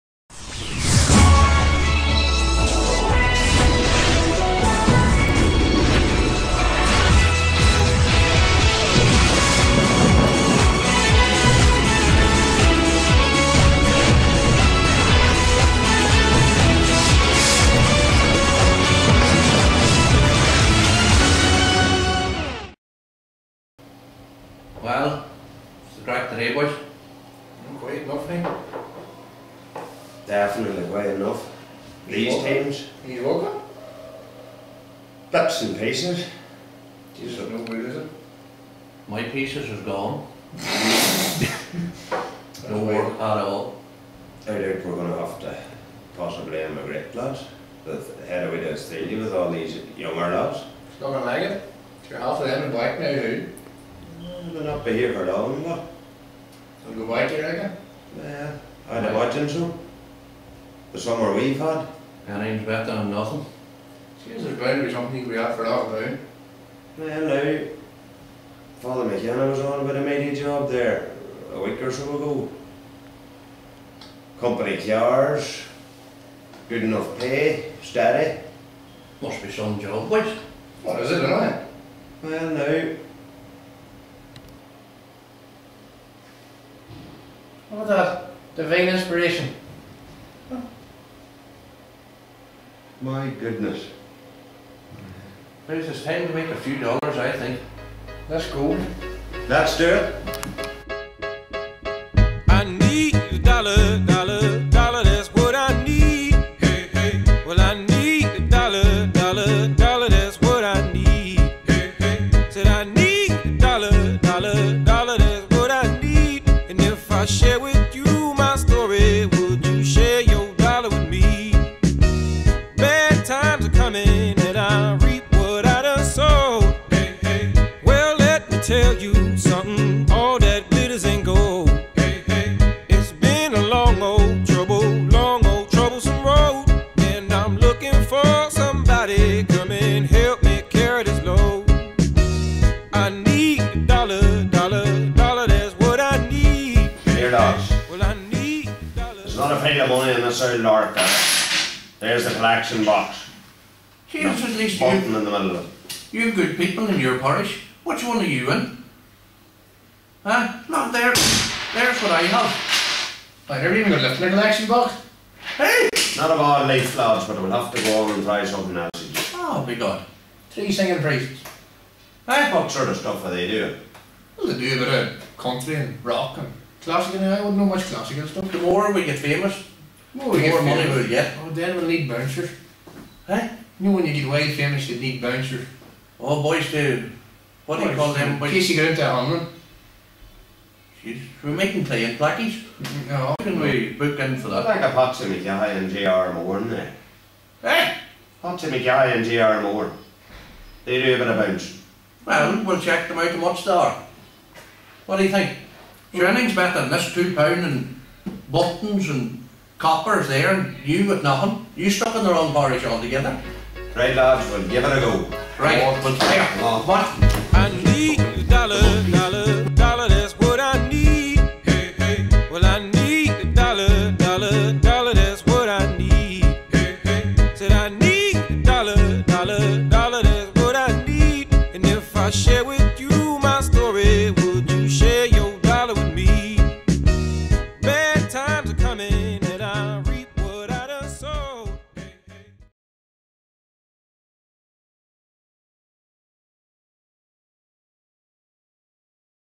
trillicks got talent - singing priests preview
ACT 1 at Trillicks got Talent on Friday 26th August 2011 featuring Michael Kelly, Padraig Tunney and Gavin McAloon. Part of the annual Rockinwiththereds festival hosted by Trillick GFC